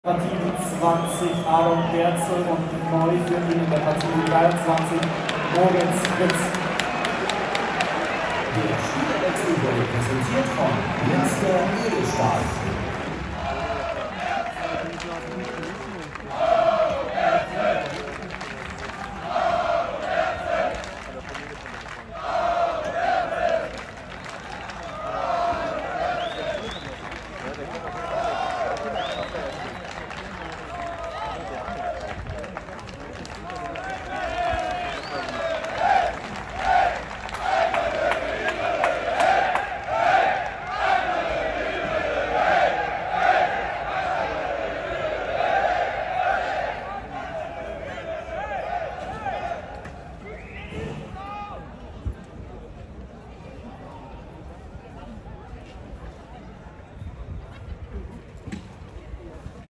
Aaron Berzel - TSV 1860 München - Viktoria Köln - bei seiner Auswechselung und nach dem Spiel
Der ehemalige Löwenspieler Aaron Berzel wird von den Fans des TSV 1860 München gefeiert